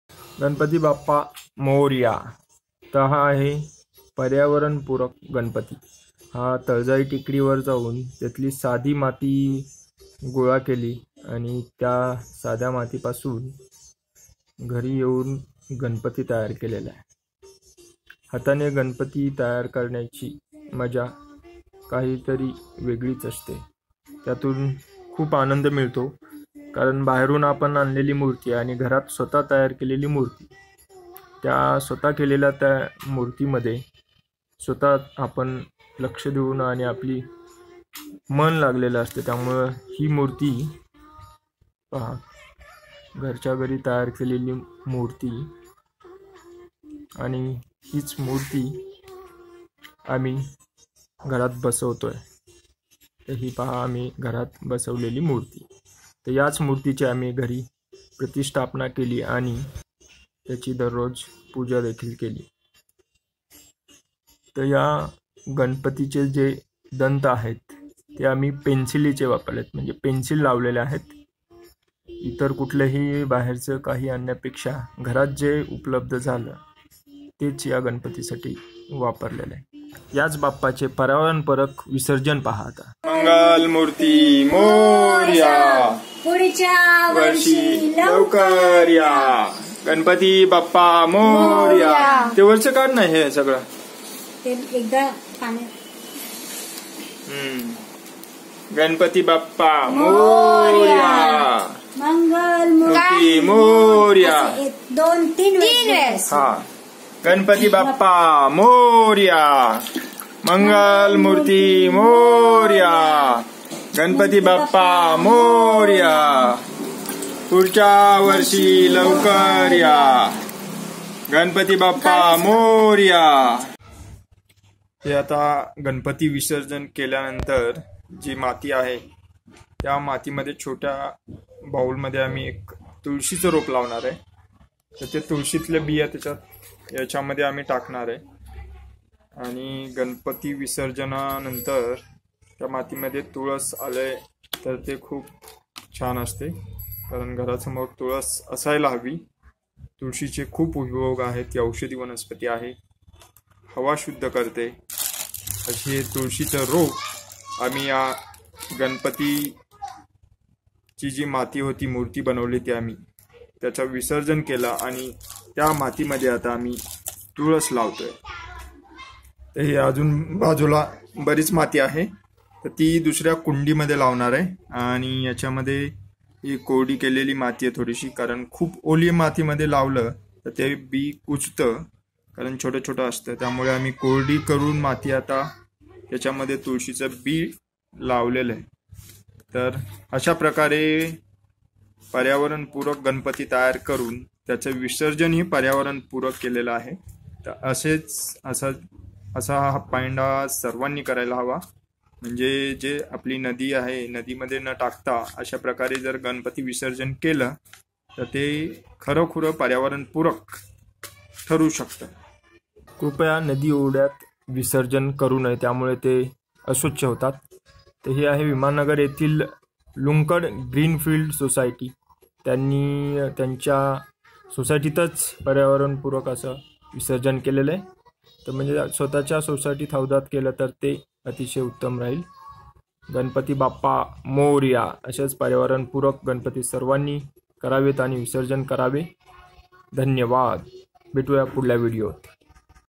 पर्यावरणपूरक गणपती / कुंडीत माती ठेवून तुळसीची लागवड/ विसर्जनही पर्यावरणपूरक/
पर्यावरणपूरक गणपती म्हणजे काय ? अनेकदा शाडूची किंवा चिकनमाती आणून गणपती बनवतो. पण ते नदी, ओढ्यात विसर्जन केलं तर पर्यावरणपूरक ठरत नाही. खरं तर साधी मातीपासून गणराय तयार करून ती माती परत निसर्गात तशीच परत केली पाहिजे. घरात अगदी तसेच गणराय आले आणि त्यांचे विसर्जन अनोख्या पद्धतीने केलं. खूप आनंद झाला. सर्वांनीच अशा प्रकारे गणपतींचे सर्वात करून निरोप द्यायला हवा. तरच निसर्ग संवर्धन होऊ शकेल. गणपती बाप्पा मोरया...